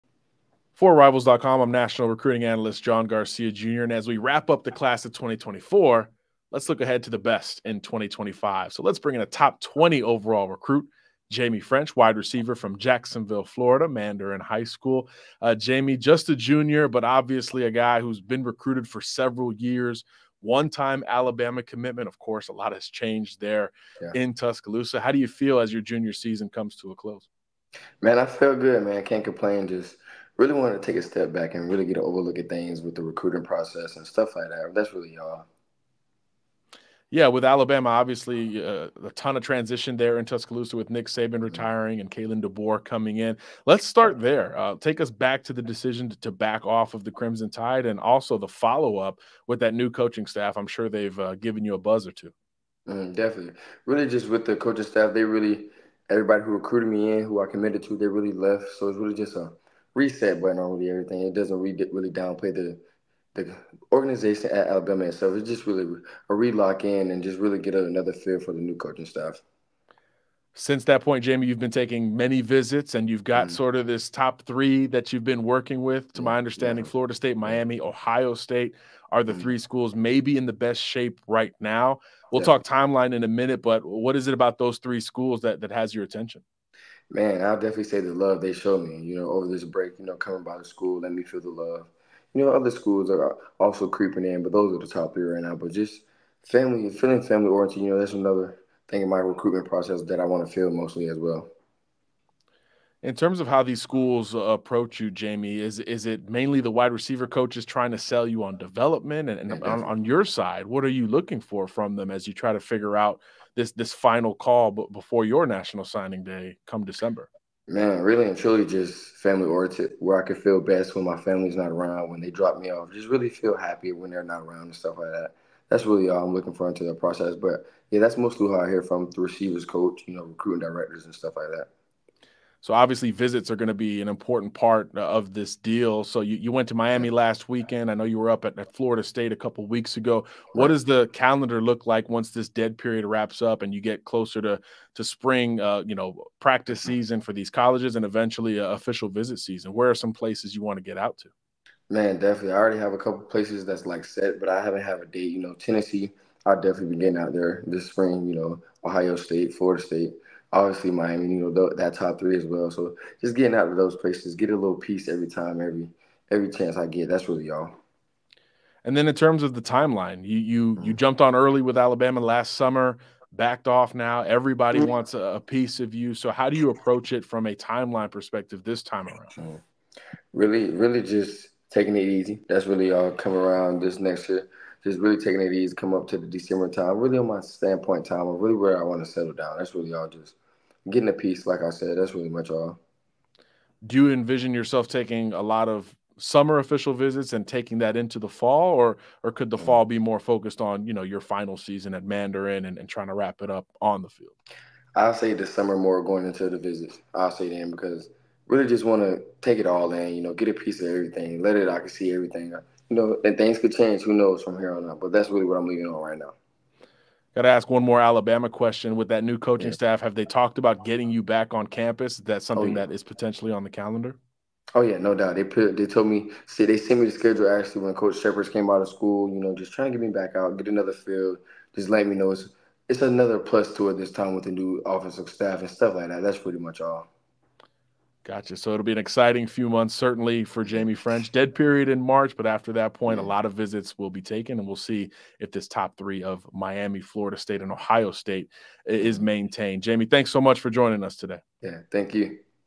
Looking ahead to 2025: four-star WR Jaime Ffrench
John Garcia Jr. catches up with 2025 wide receiver Jaime Ffrench to break down where his recruitment currently stands.